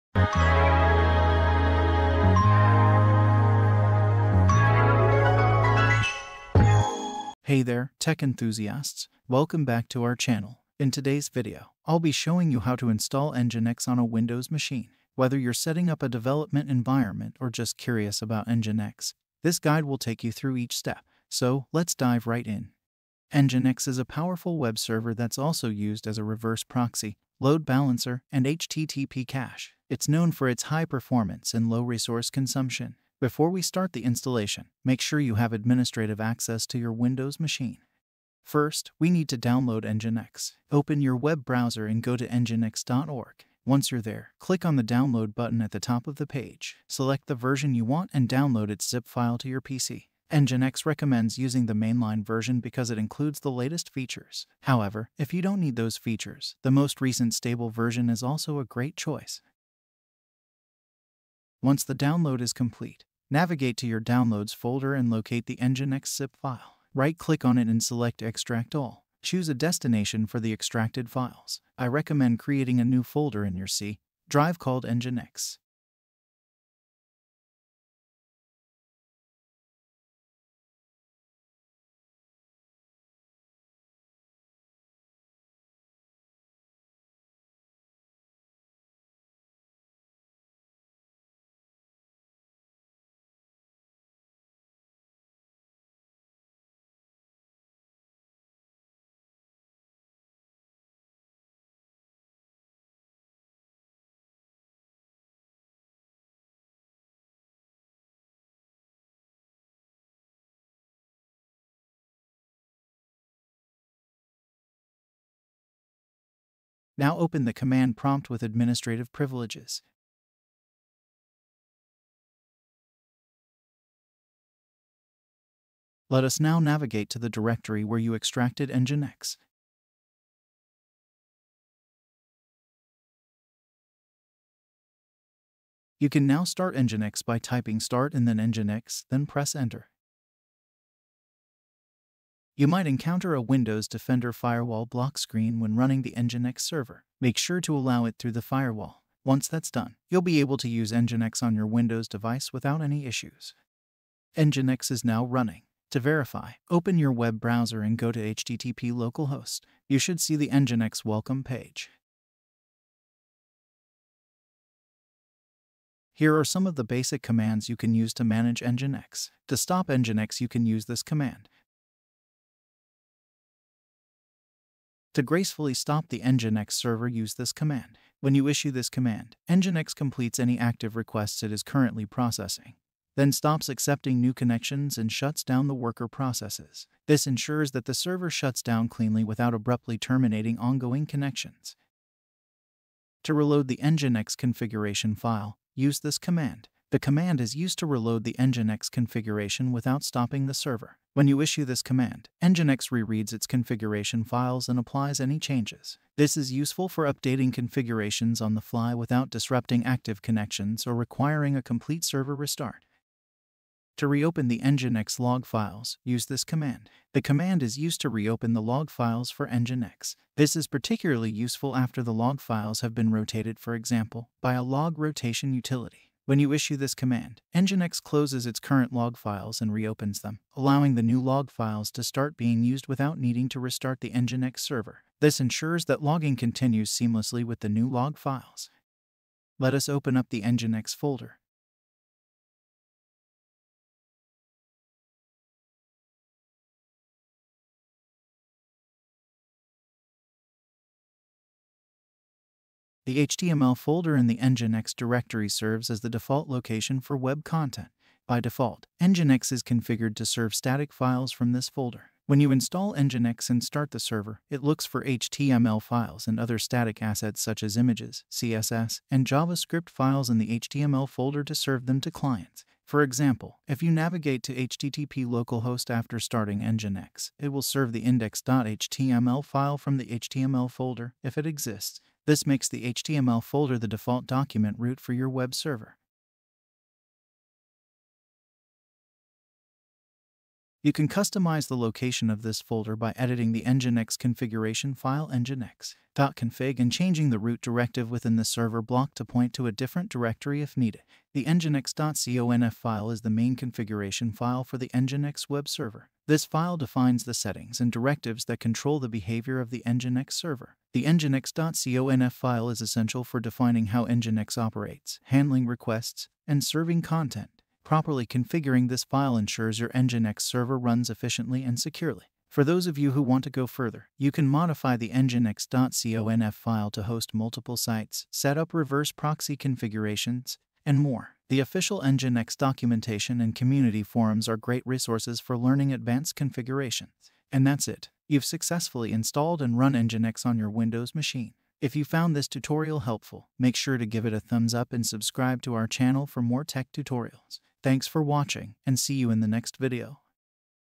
How to Install Nginx on Windows 11/10: Easy Guide
Looking to set up Nginx on your Windows 11 or 10 system? You're in the right place! In this step-by-step tutorial, we'll walk you through the straightforward process of installing Nginx, the powerful web server and reverse proxy, on your Windows machine.

Nginx offers lightning-fast performance and is widely used by developers and administrators alike to efficiently serve web content. Whether you're a seasoned pro or just getting started with web servers, this tutorial is designed to make the installation process smooth and hassle-free.